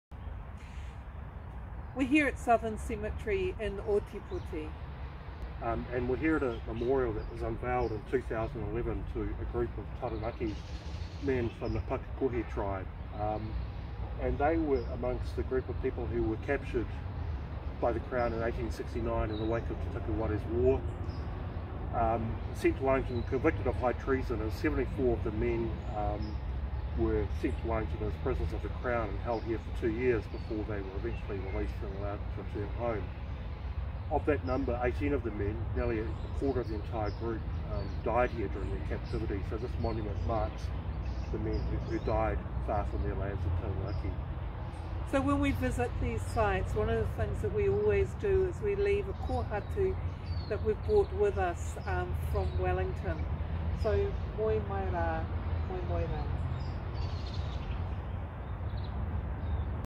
The Pakakohi Prisoners in Dunedin
In November 1869, 74 men from the Pakakohi tribe in Taranaki were sent to Dunedin to serve hard labour, having previously been convicted of high treason for supporting Titokowaru’s resistance to the confiscation of their lands.

In 1872 the surviving men were released and allowed to return home. But in the harsh and unfamiliar conditions, 18 of the men – nearly a quarter of the total number – had died in captivity, far from their homes.

In 2011 a memorial was unveiled in Dunedin’s Southern Cemetery in memory of these men, who had been buried in unmarked graves. In this latest video from our project, Professor Joanna Kidman and Dr Vincent O’Malley visit the memorial.